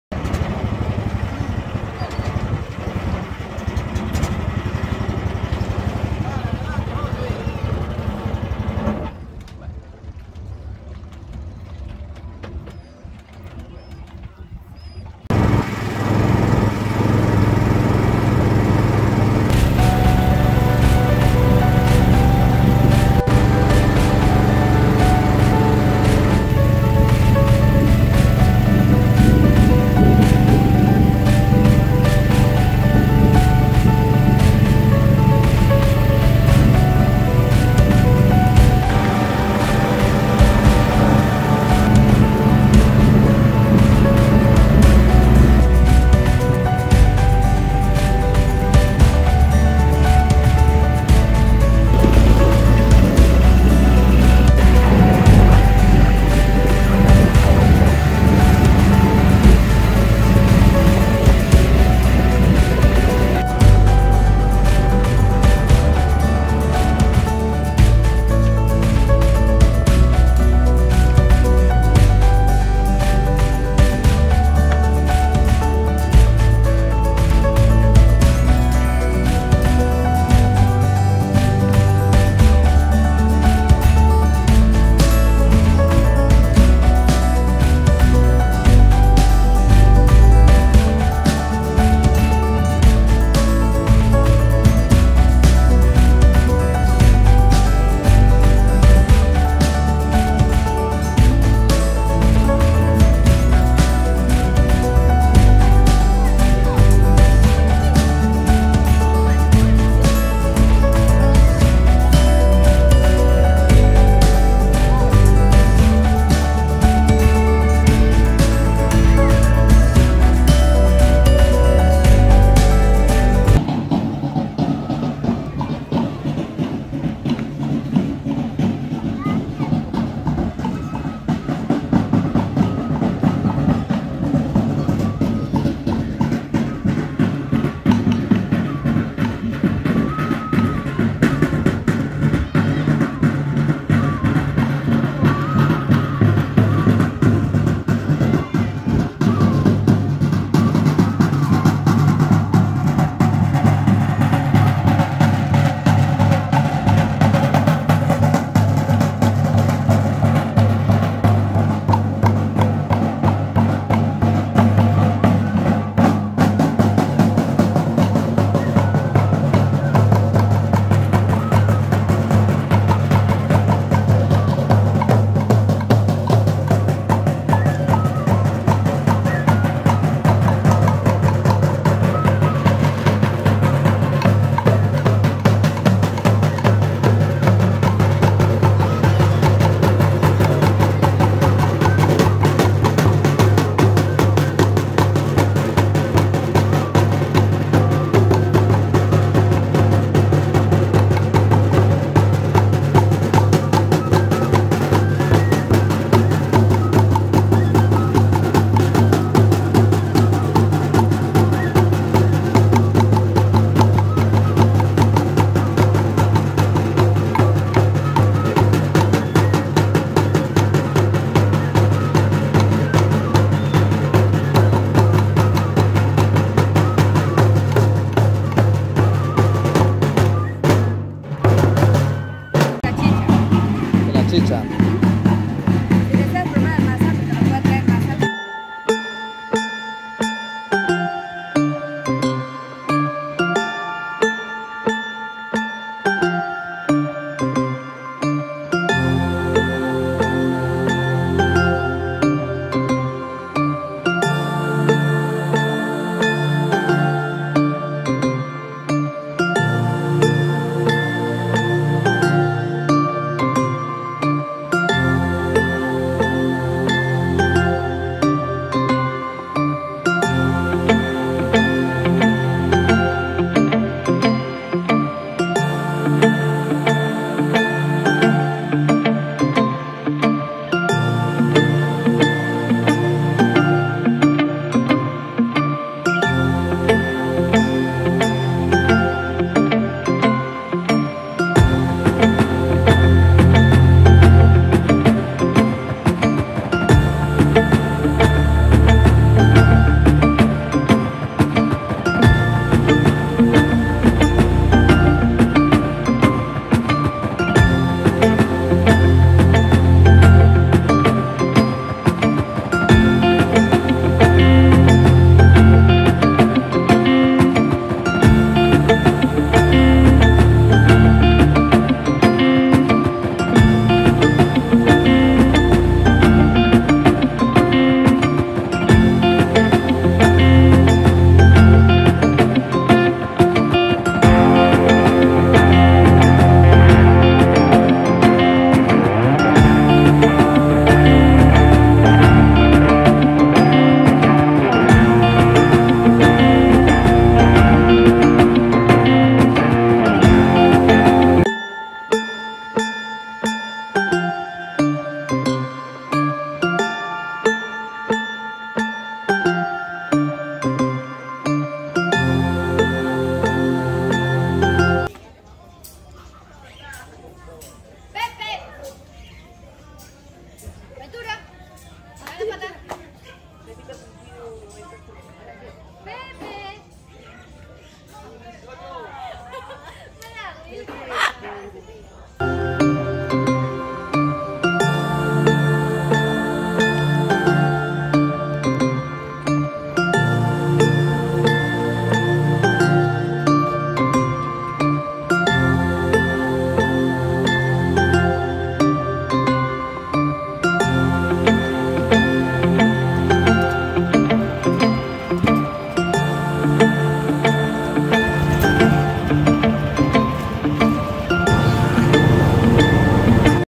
VLOG # 3 Caserio 11 de Agosto - Yarinacocha
un publito estupendo con su gente tan amable, vitalos ellos los recibiran de la mejor forma con su masato🙌🙌